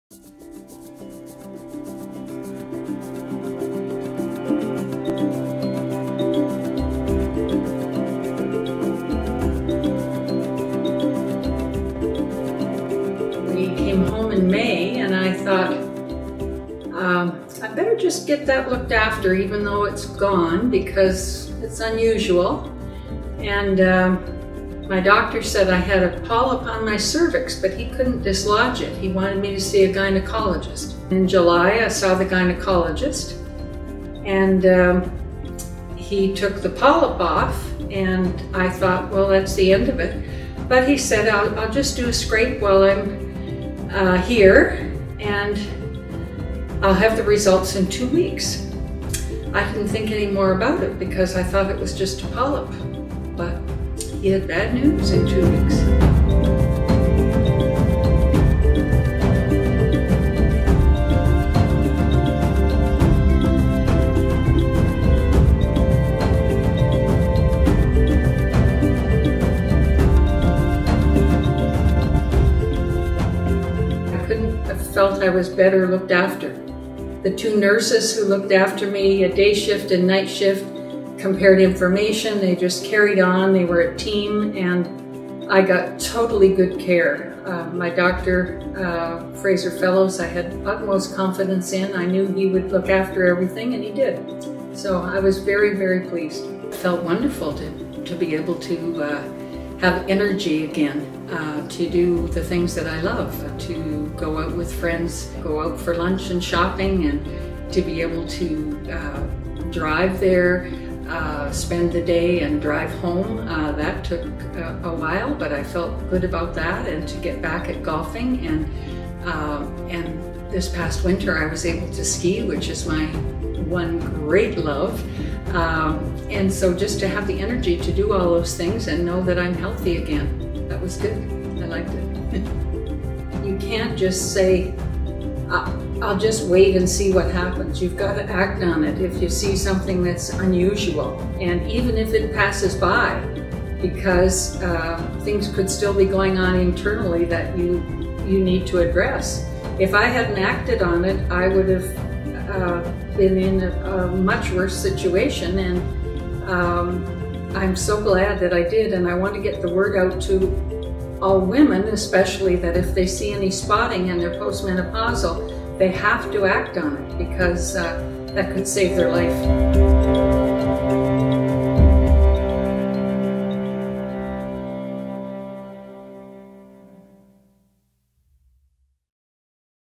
Lorraine's experience with endometrial cancer and the importance of acting quickly
While on vacation Lorraine Jardine experienced occasional spotting, unusual in post-menopausal women, and made sure she saw her doctor right away when she returned home. Lorraine discusses her experience with endometrial cancer and the importance of acting quickly.